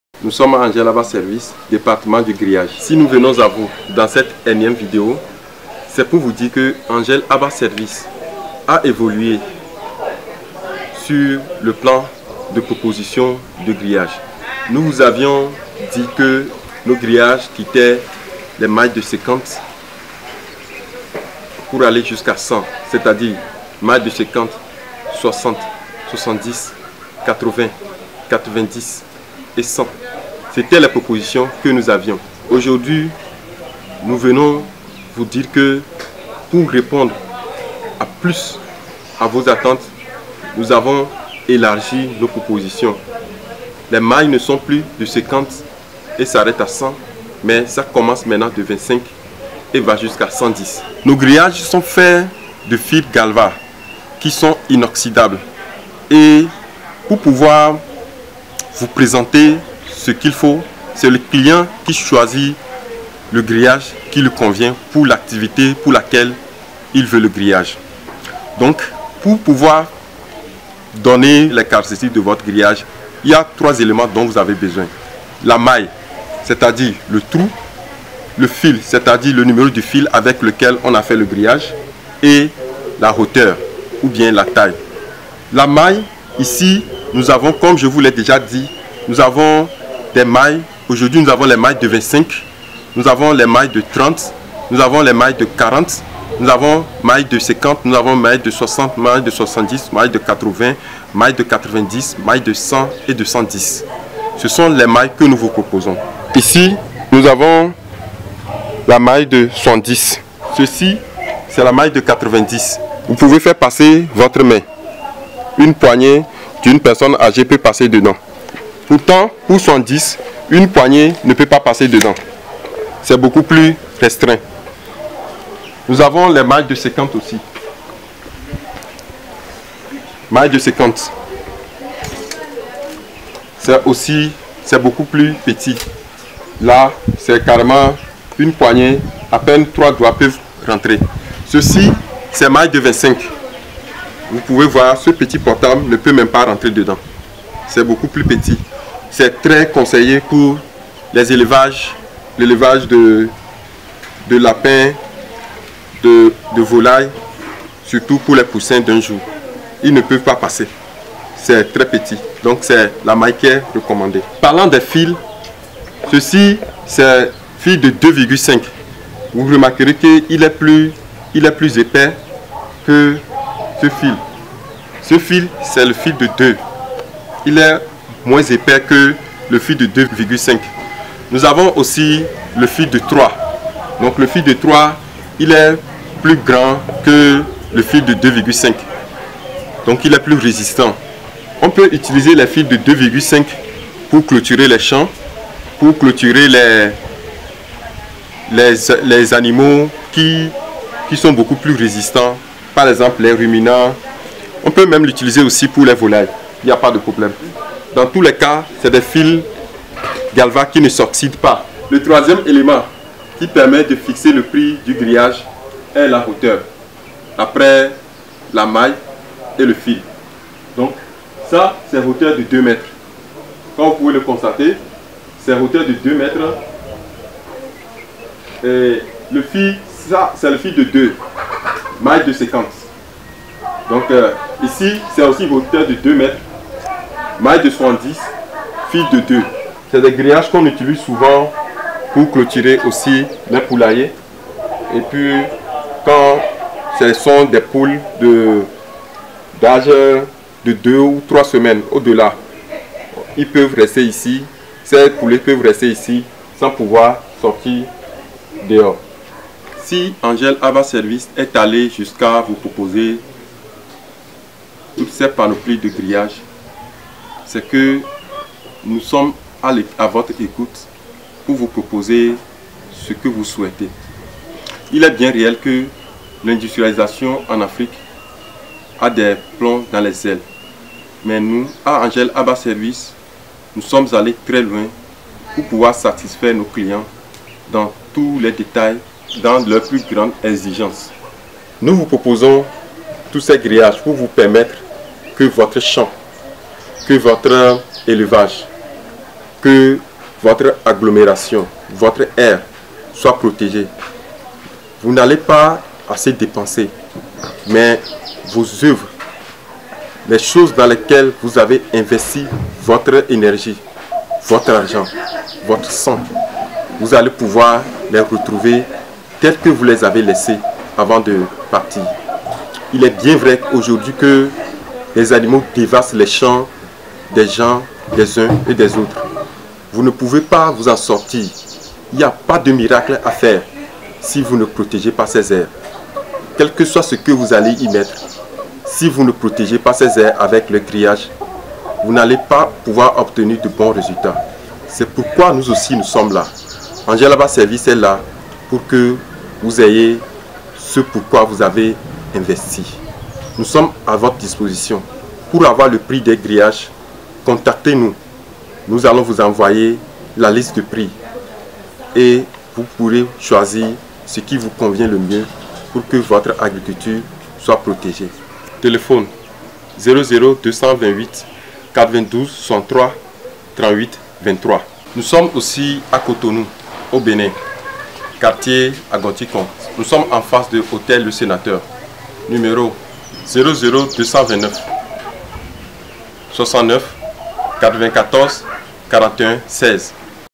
Grillage de protection
Nous produisons et vendons des grillages pour clôture en sécurité
Je propose vous envoyer la liste des prix et des différents modèles
Il faut dire que le prix du rouleau de grillage varie selon 3 principaux critères:
1. Le numéro du fil ( n° 2, 2,5 et 3)
Plus le numéro est petit, plus le fil est fin
2. La maille
Les dimensions des losanges qui forment le grillage. Plus la maille est grande, plus le grillage est moins cher. Le choix de la maille est relatif à l'utilisation qu'on fera du grillage.
3. La hauteur
Les rouleaux de nos grillages sont de 25m en longueur. La hauteur est la largeur du rouleau.
Quel grillage voulez vous?